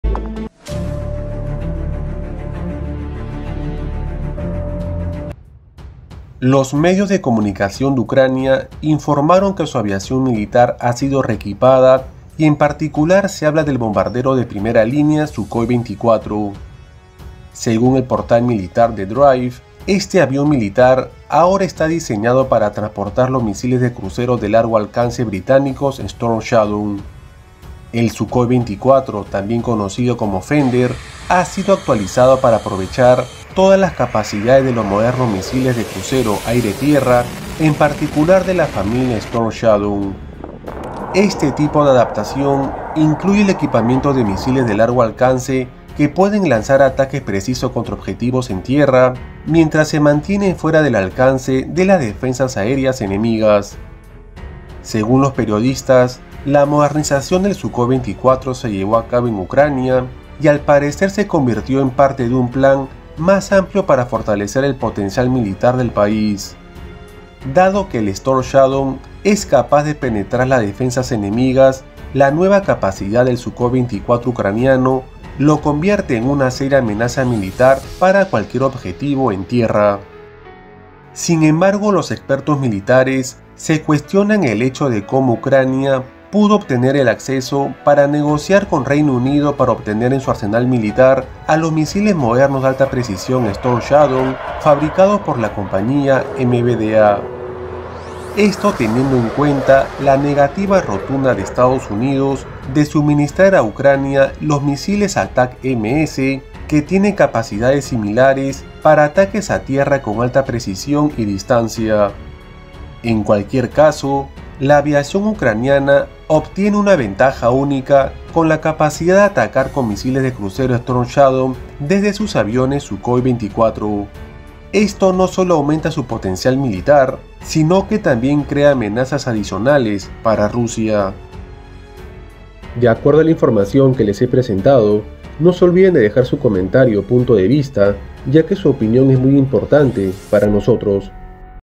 🔴ÚLTIMA HORA : RUSIA Alerta ante Bombarderos de UCRANIA equipados con MISILES de largo Alcance
Esta Información viene desde Ucrania , esperamos sea de su interés. Buen día.